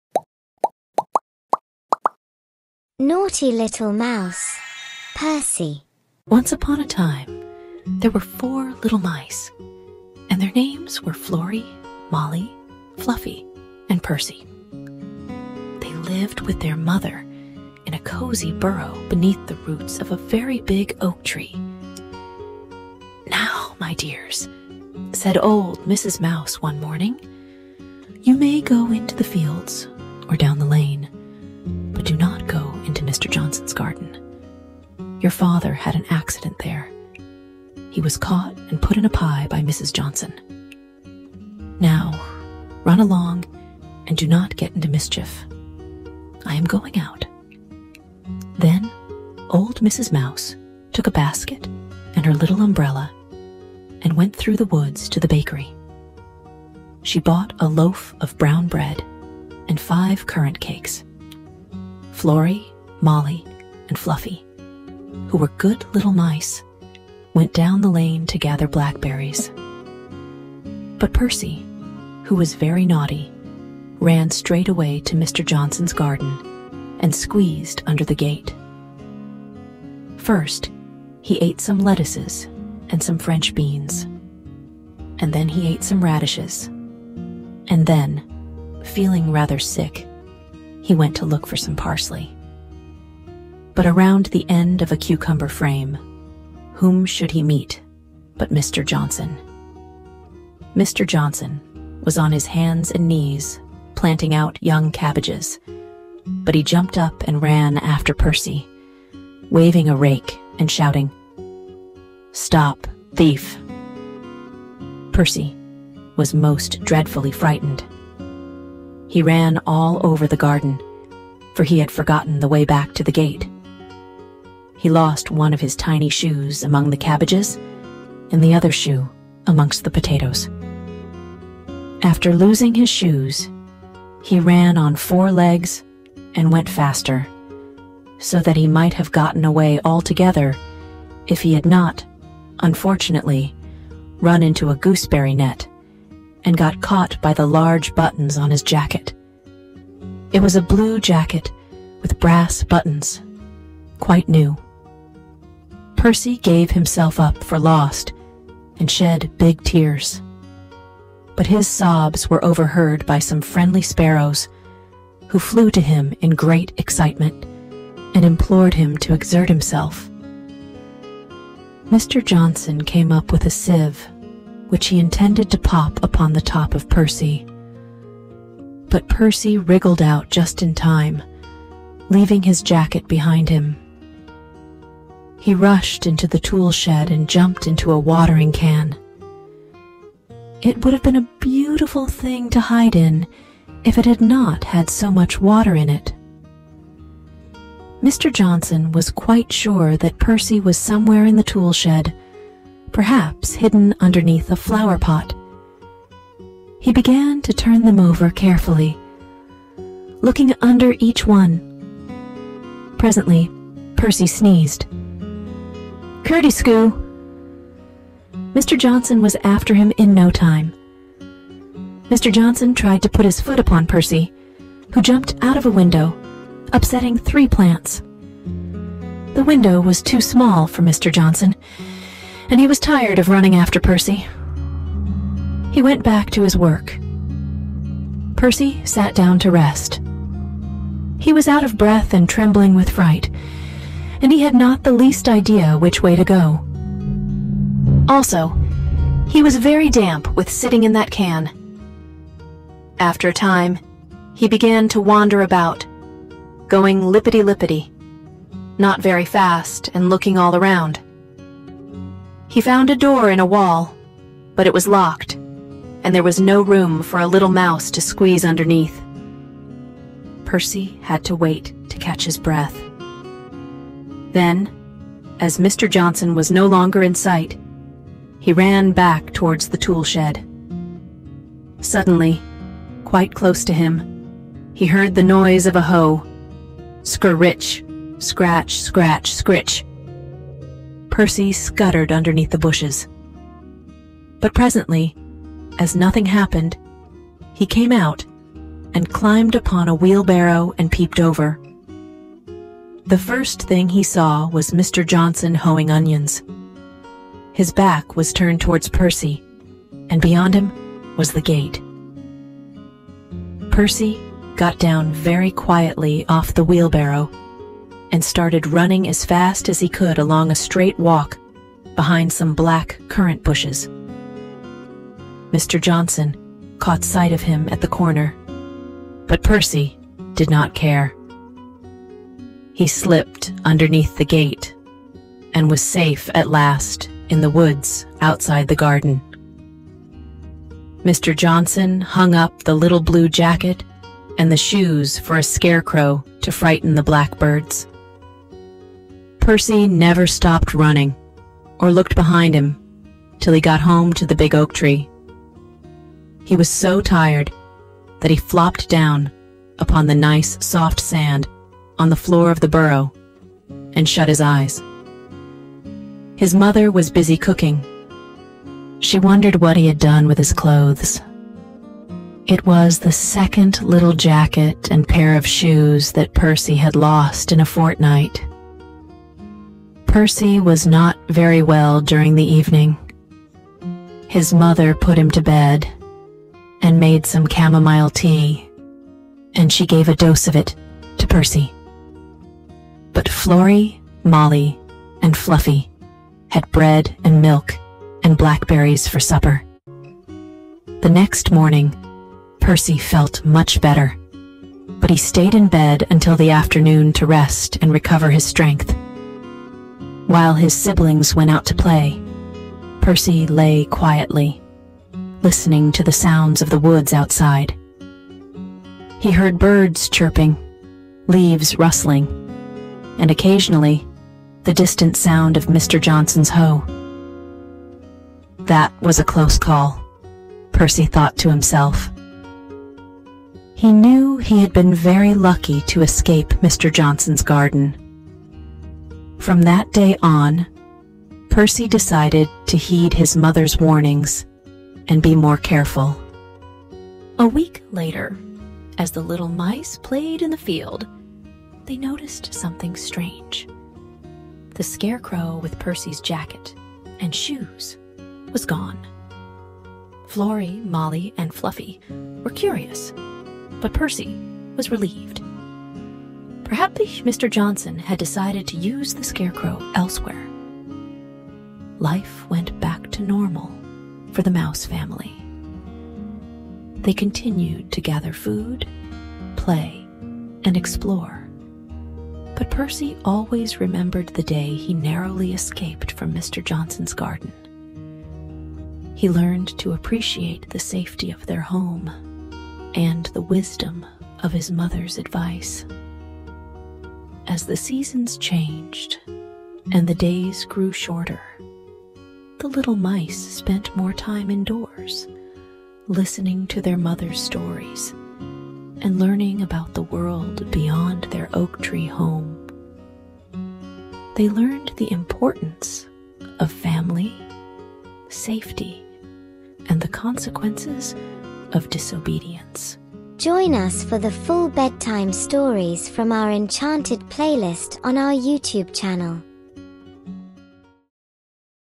[EP1] Naughty little mouse Percy | Chap 1: Naughty Mouse get caught | Bedtime stories | Cozy Sleepy
Percy the naughty mouse and stories with his lovely siblings. Despite warnings, mischievous Percy took adventures in the village and turned things up. He learns the importance of heeding his parents' advice and the value of family and safety.